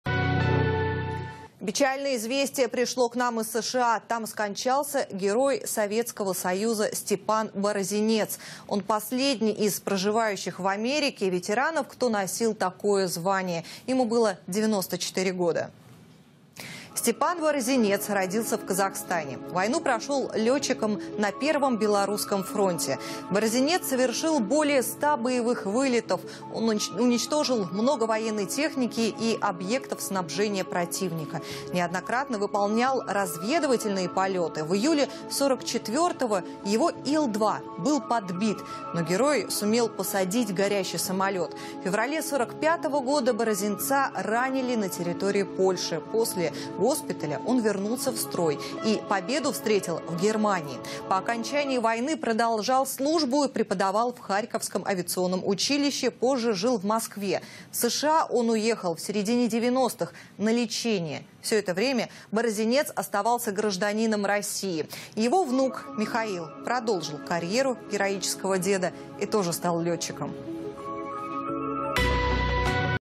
В США скончался Герой Советского Союза Степан Борозенец
Борозенец - прославненный летчик Великой Отечественной Войны